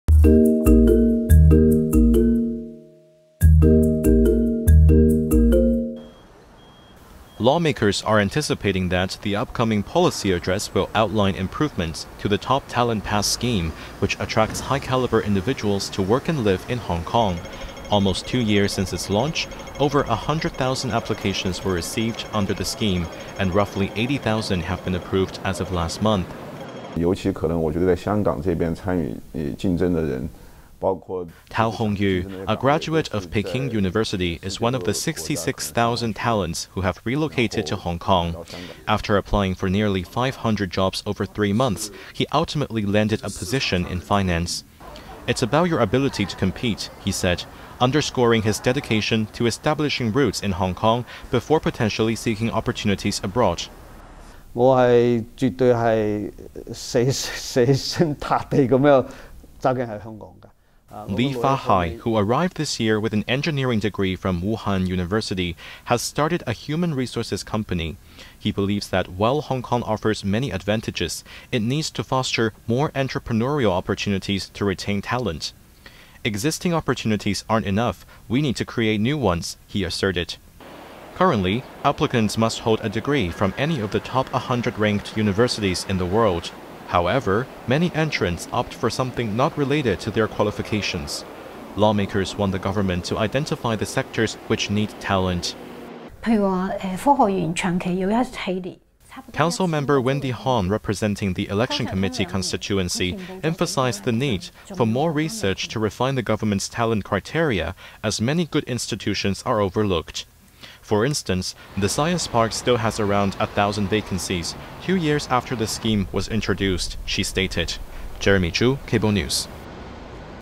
Talent Pass Scheme Needs Fine-tuning
Lawmakers are anticipating that the upcoming policy address will outline improvements to the Top Talent Pass Scheme which attracts high-calibre individuals to work and live in Hong Kong.